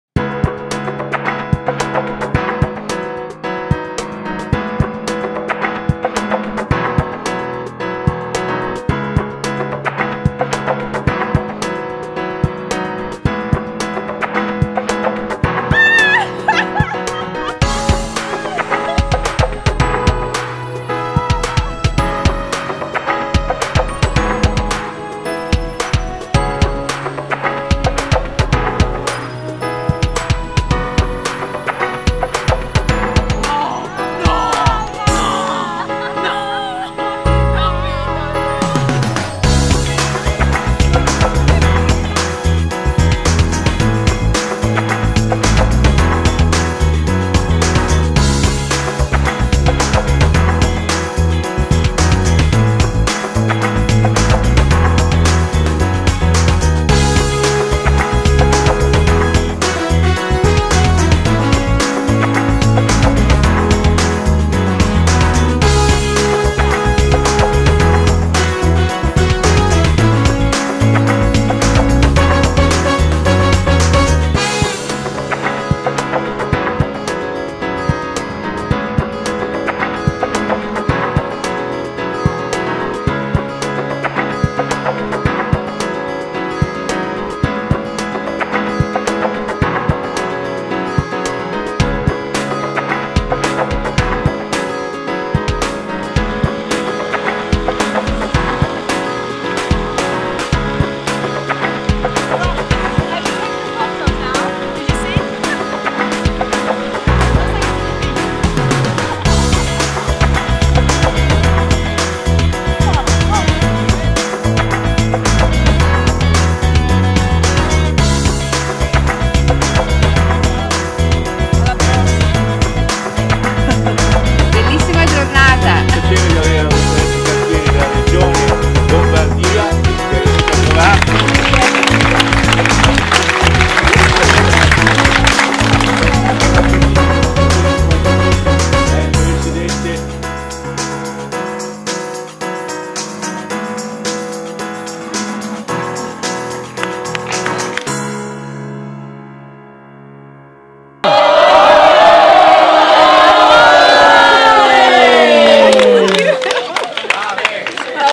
GOLF È DONNA 2020 / FRANCIACORTA GOLF CLUB
Una giornata di golf speciale dedicata alle donne, nella splendida cornice del FRANCIACORTA GOLF CLUB, con 25 neofite al fianco in squadra di altrettante giocatrici.  Il progetto GOLF È DONNA si pone l’obiettivo sia di avvicinare nuove potenziali golfiste, sia di fidelizzare chi gioca regolarmente a questo sport.